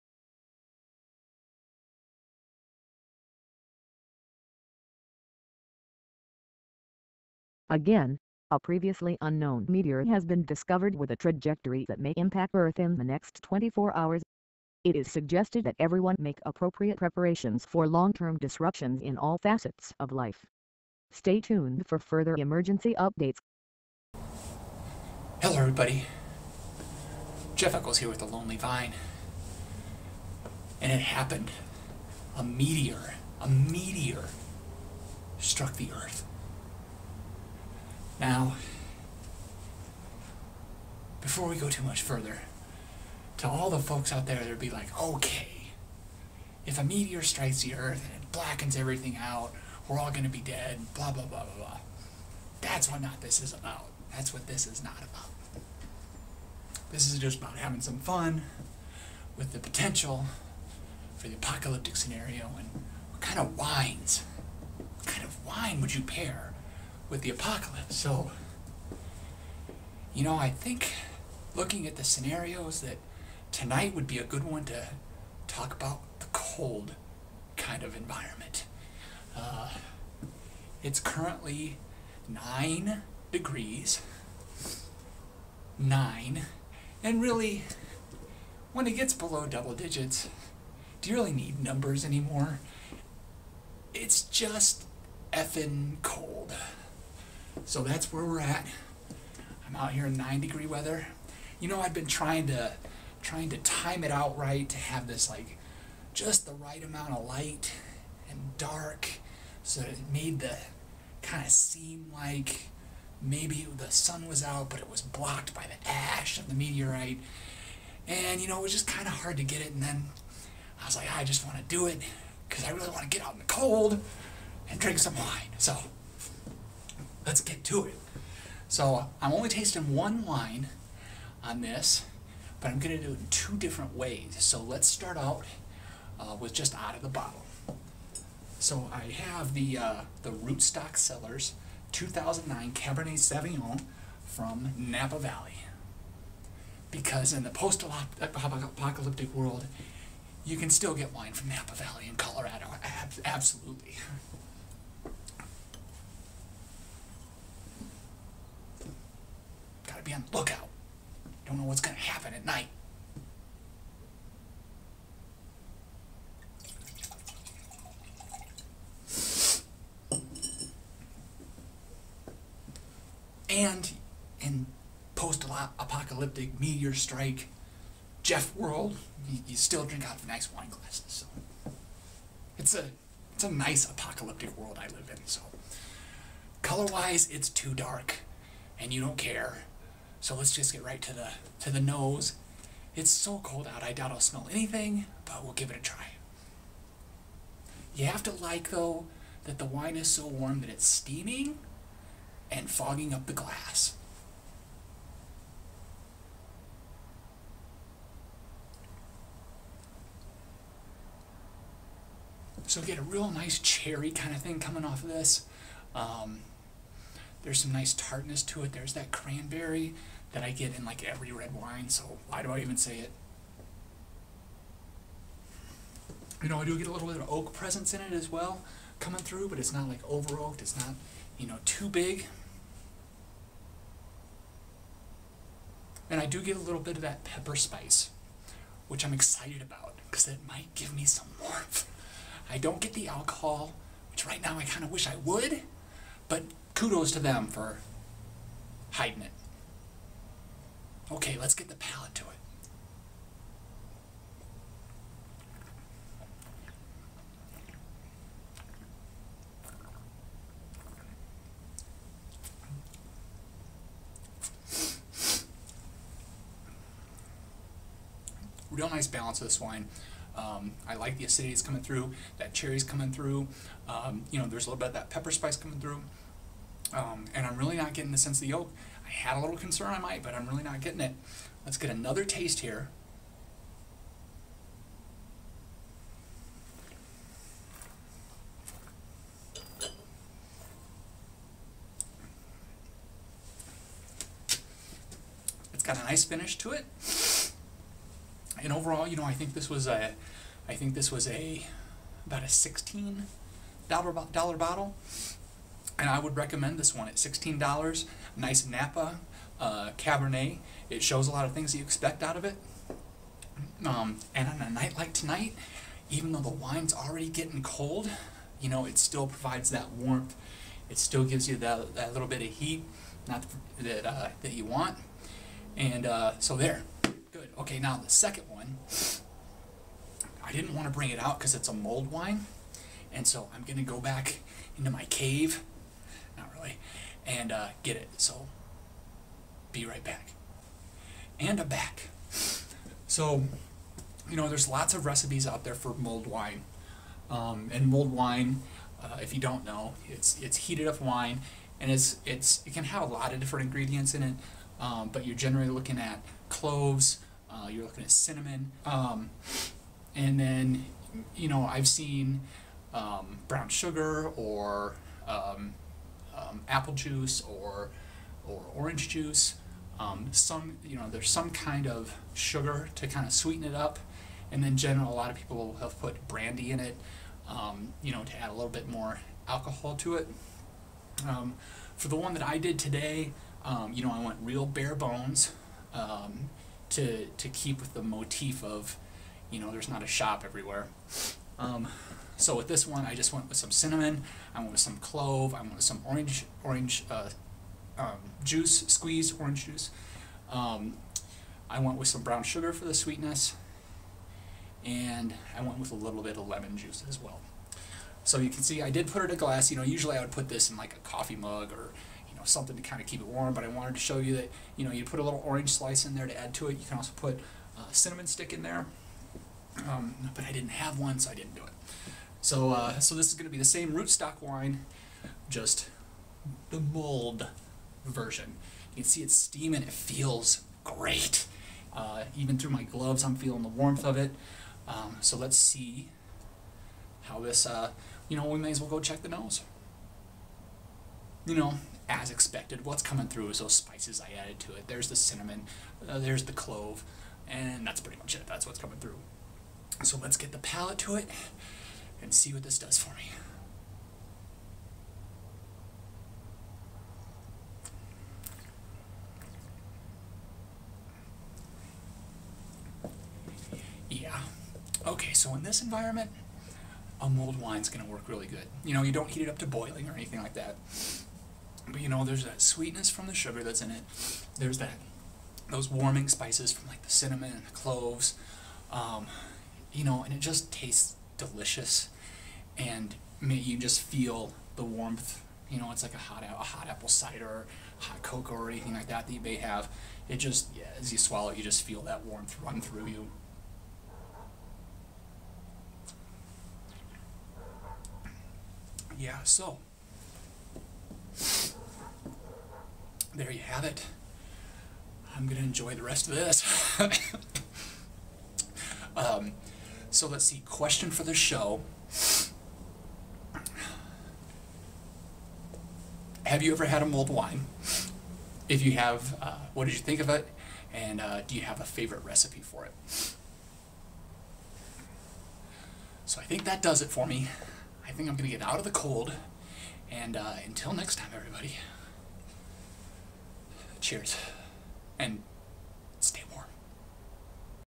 Wines of the Apocalypse - Meteor Strike - Episode 73
I venture out in the cold to find out what wine will work best when the temperature outside is frightful.